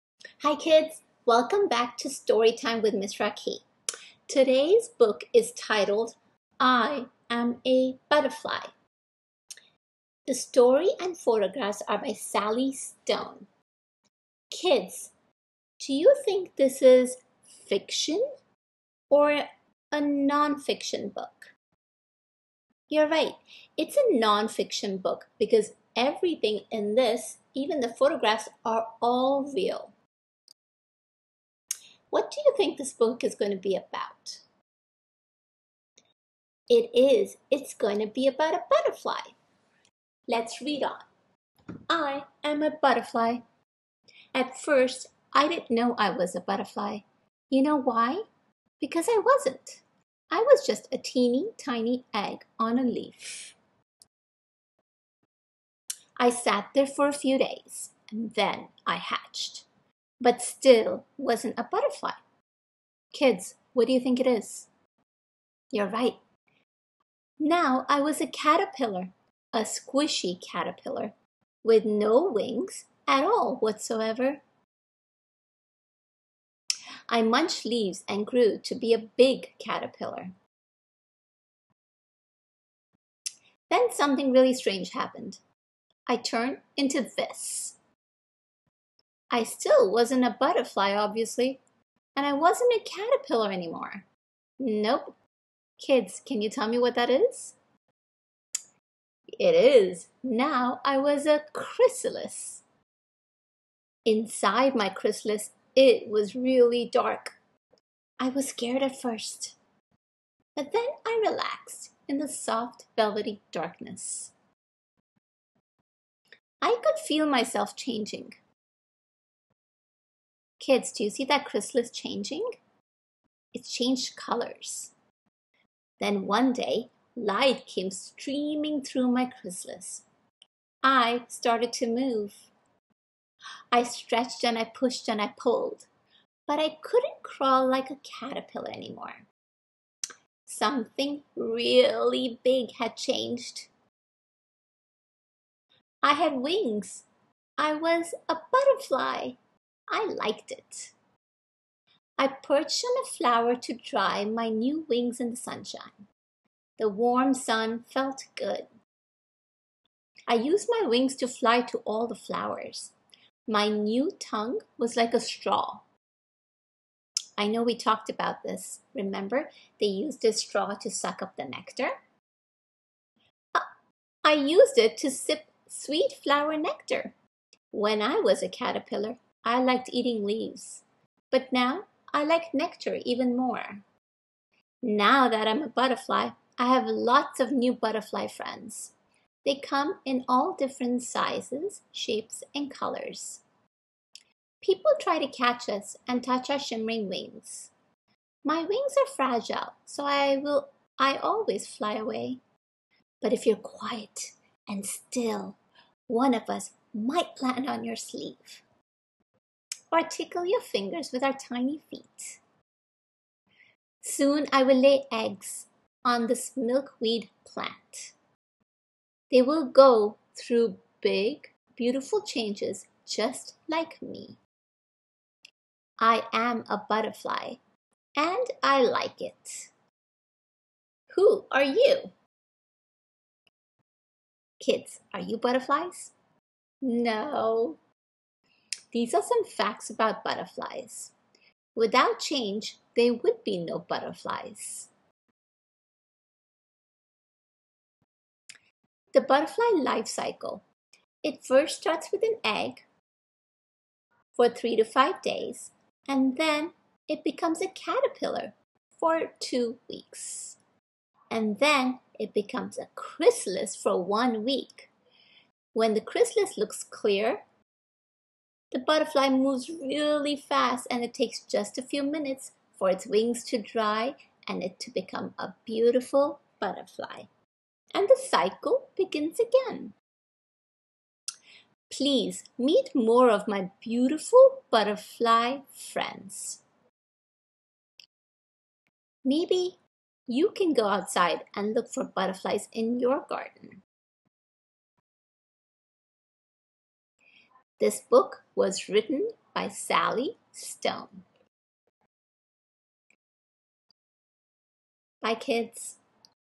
Okacoteach_ Storytime I am a Butterfly_5.15
Ms. Rakhee reads a non-fiction book 'I am a Butterfly' by Sally Stone